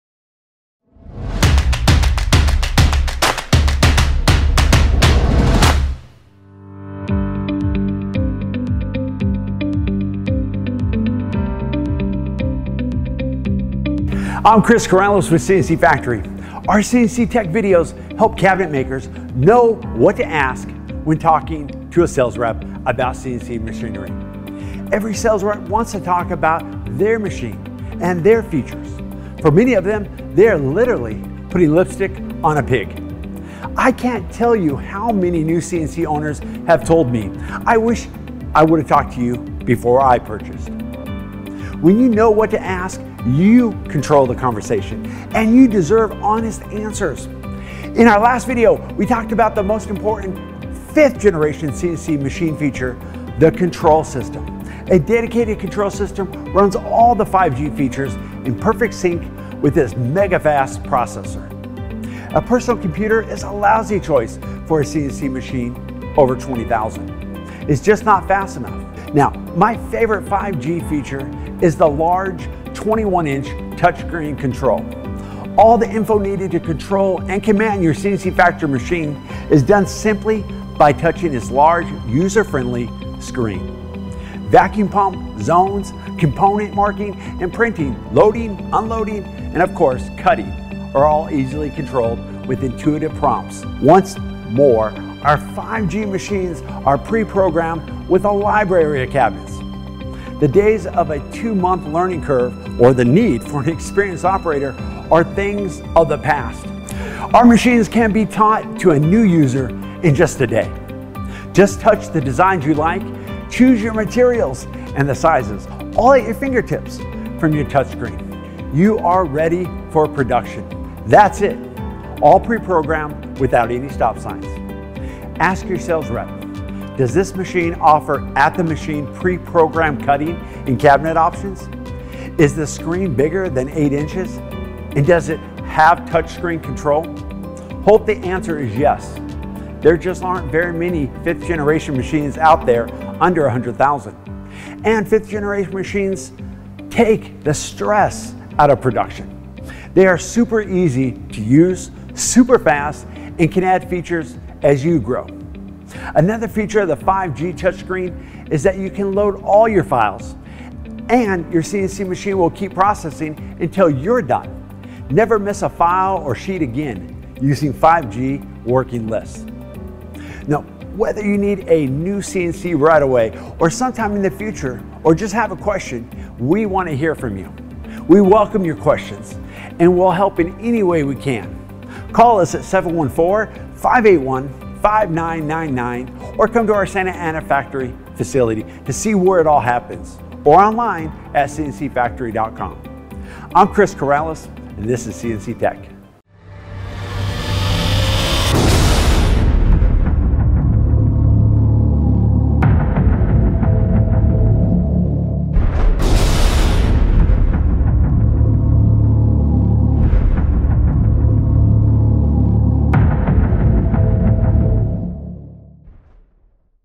Is your CNC controller touch screen and over 20"? Ask before buying-- CNC Tech/CNC Factory
Make sure you don’t get a small handheld or simple CNC control under 8”. You will quickly learn the limitations of a cheap-import CNC controls once you start working with multiple processes. Having a large interface makes it easy on the operator and minimizes errors. Choose 5G CNC with a screen over 20” and is by the machine, so you can select cabinets, cuts, and make edits on the fly.

Want to see all our router features at a glance? Check this out
Find us anywhere with TheCNCFactoryExperience